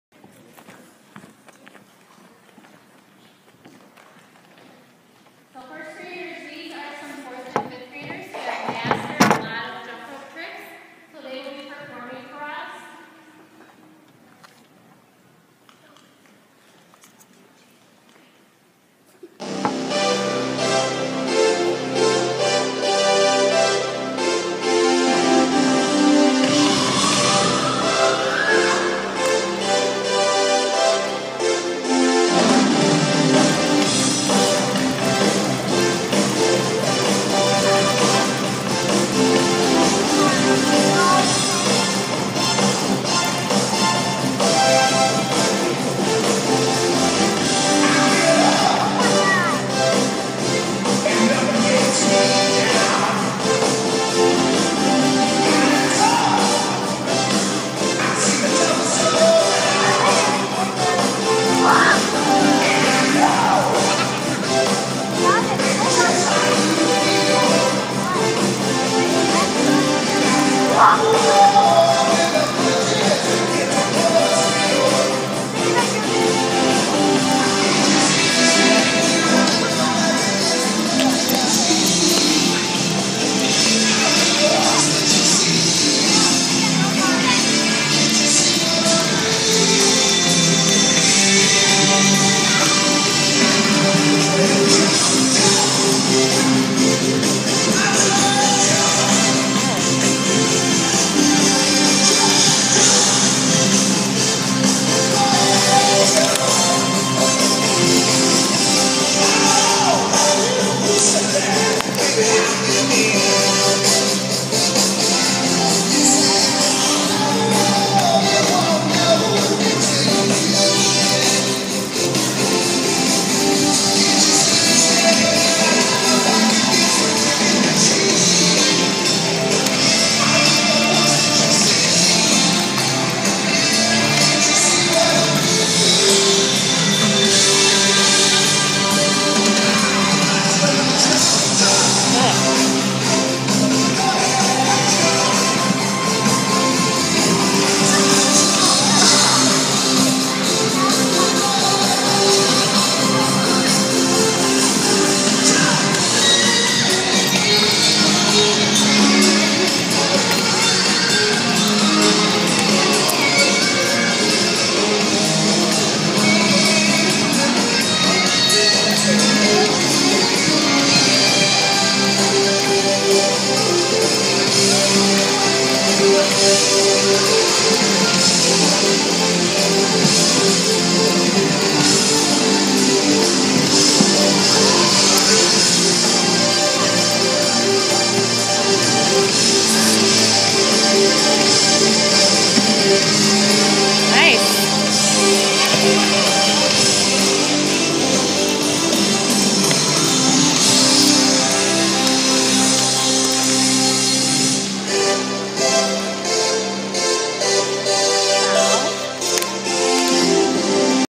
Jump rope practice performance #2
I know this is not the best quality!  Just trying to get all the jumpers on tape for parents to see.  I was holding up sings and trying to tape at the same time!